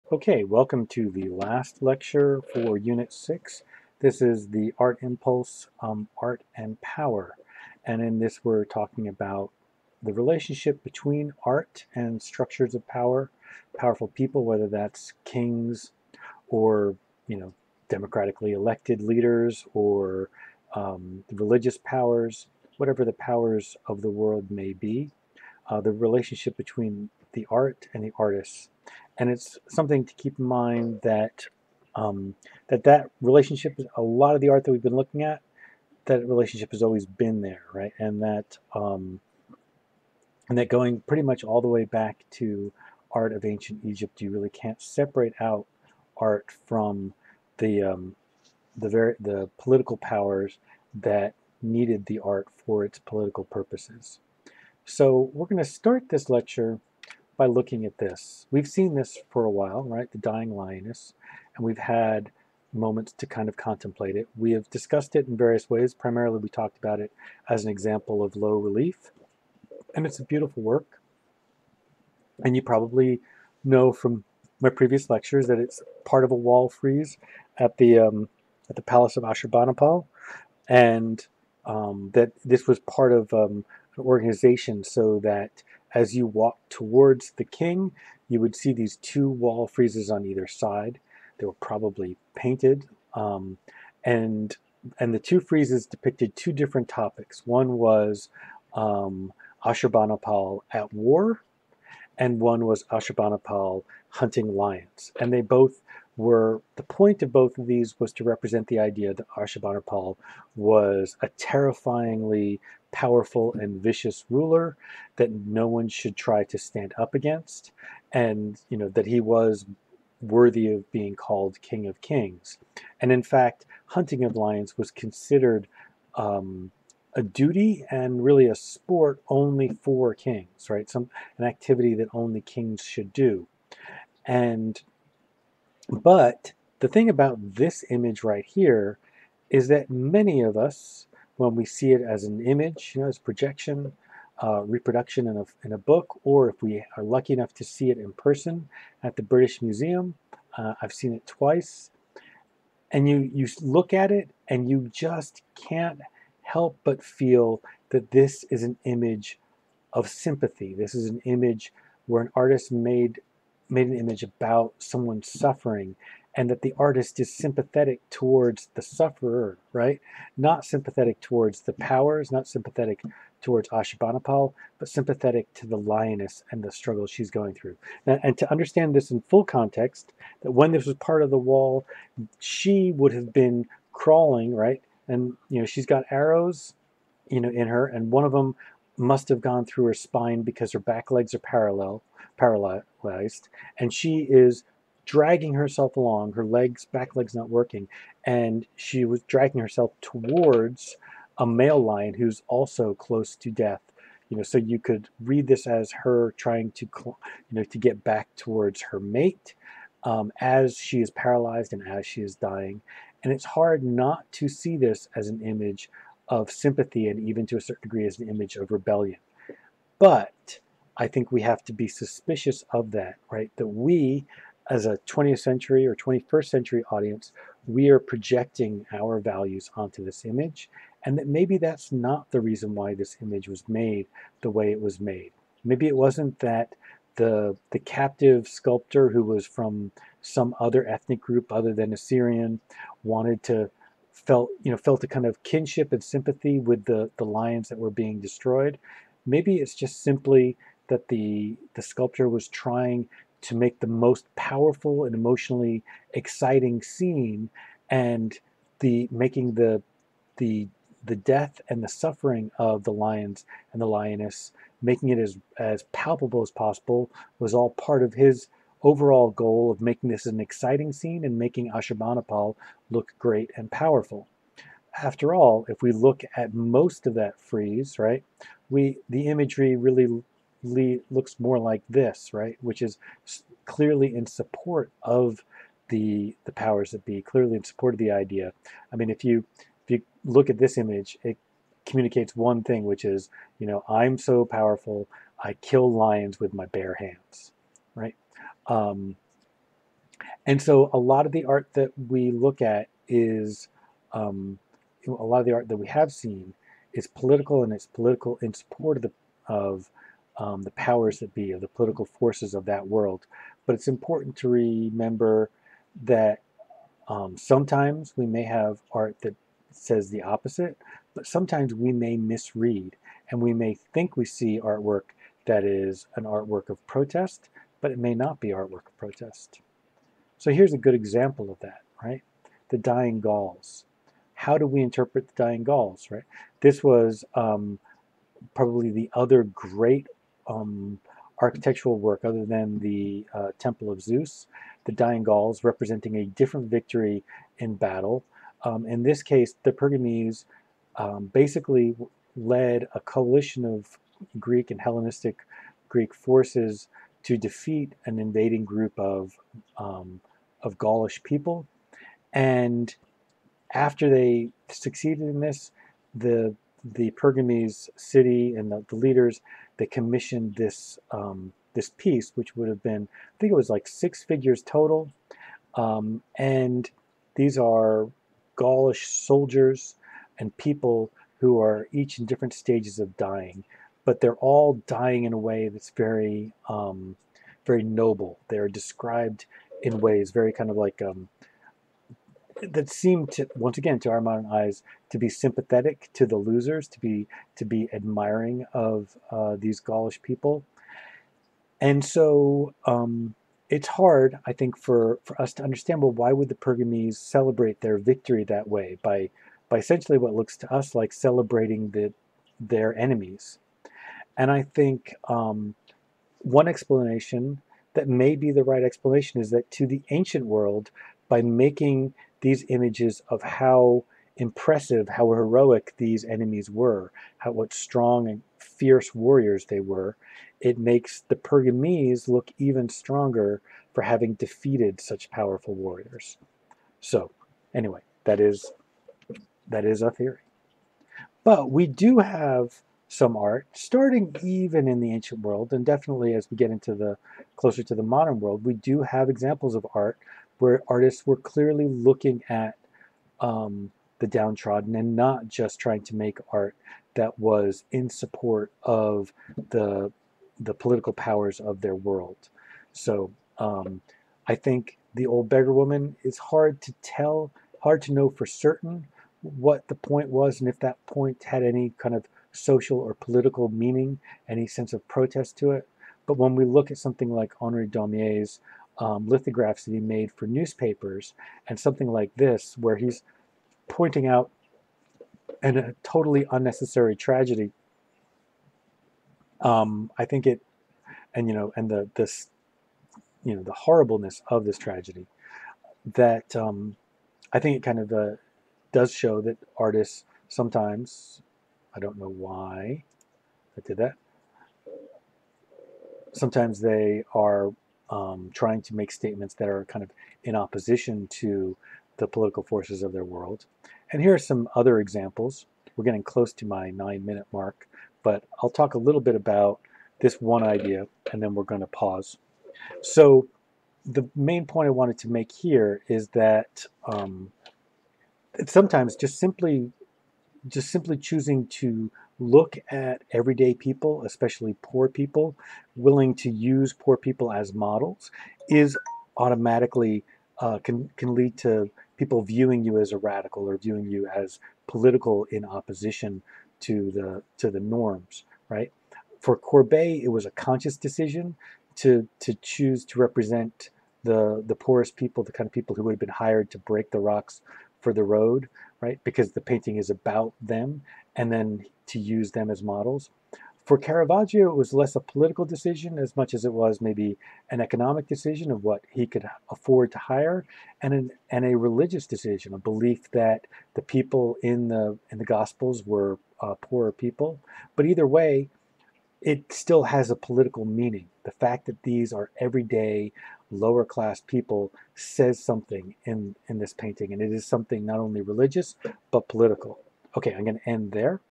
SU20 Unit 6, Art and Power: The Political Impulse, Part 1
This video is for the MUW students in the online sections of art appreciation during the summer of 2020.  All others are welcome to watch as well.

This video focuses on the relationship between art and political power and asks, "Can any art be truly anti-establishment?"